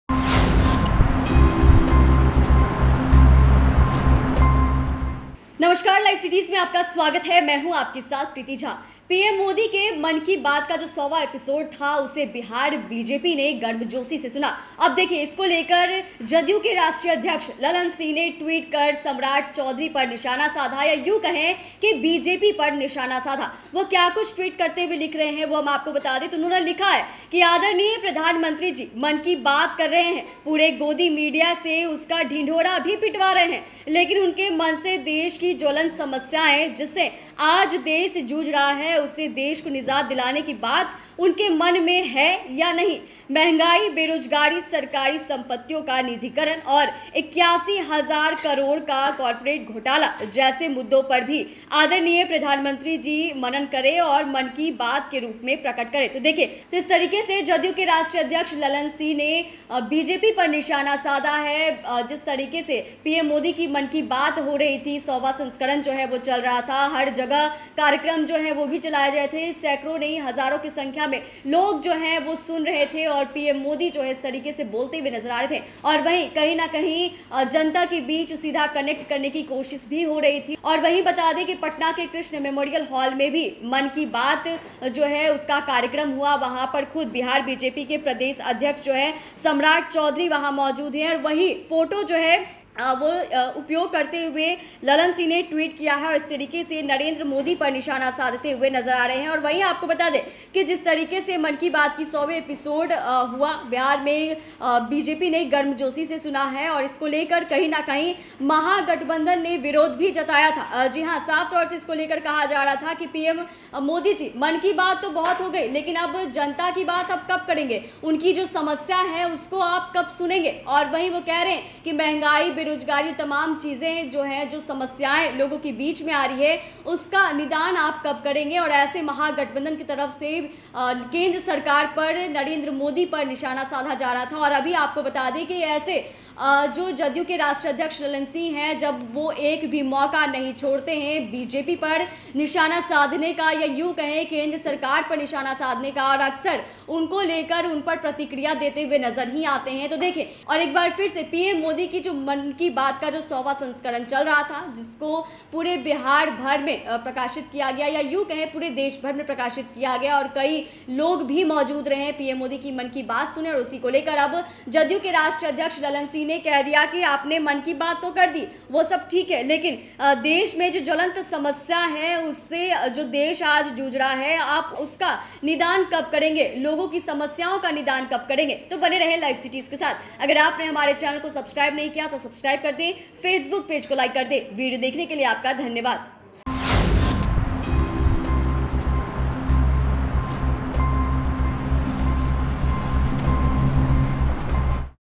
Mann Ki Baat पर बोले Lalan Singh, देश को इन मुद्दों से निजात दिलाने की बात उनके मन में है या नहीं..?
Mann Ki Baat पर बोले Lalan Singh, देश को इन मुद्दों से निजात दिलाने की बात उनके मन में है या नहीं..? | Bihar News | LiveCities